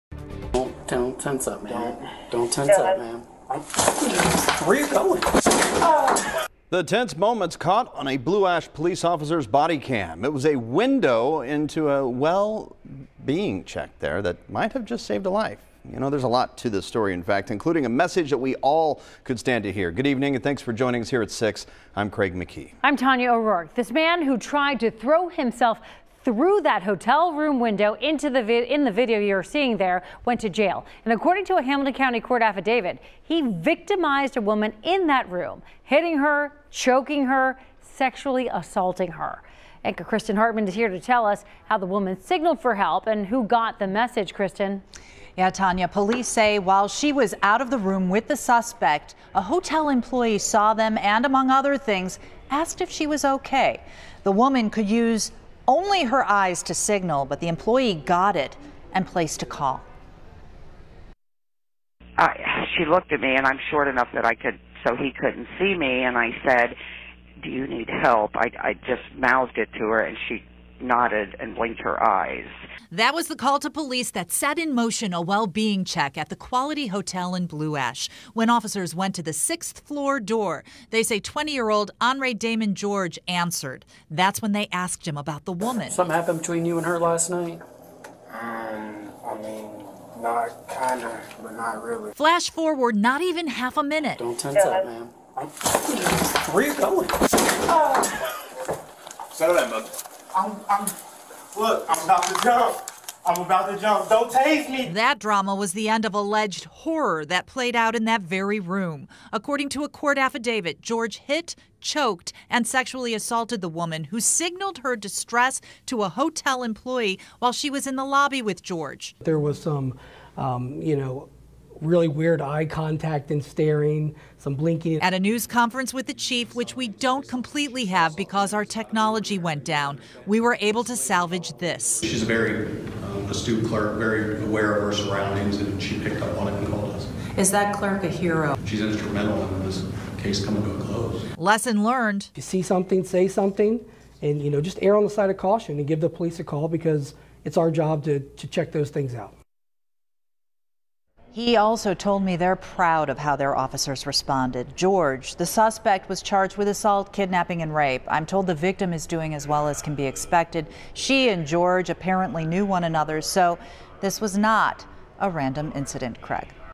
"Suspect tries to jump out window in fleeing from the cops, bounces back"
Watch "Suspect tries to jump out window in fleeing from the cops, bounces back"